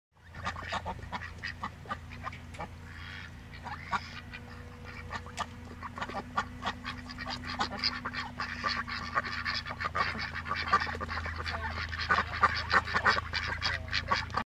Duck Herding Activity
Duck Herding is a fun and sometimes challenging activity gently herding ducks around an obstacle course. Once you learn the basics, you can work with highly trained dogs to help you.

Here the ducks are taking a break and generally kicking back.

This activity is suitable for teambuilding, hen parties, family celebrations, company fun days and village fairs.